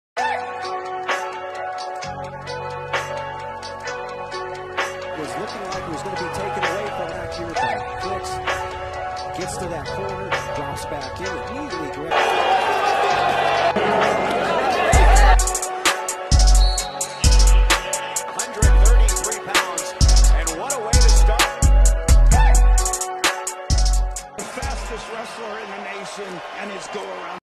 ROMAN BRAVO YOUNG NAV NCAA CHAMPION
Had fun creating highlight Reels for each champion this year.  Full Playlist of all my NCAA Coverage

I actually took Daton Fix in this bout but RBY proved his skill, hard work, and desire to be the champion!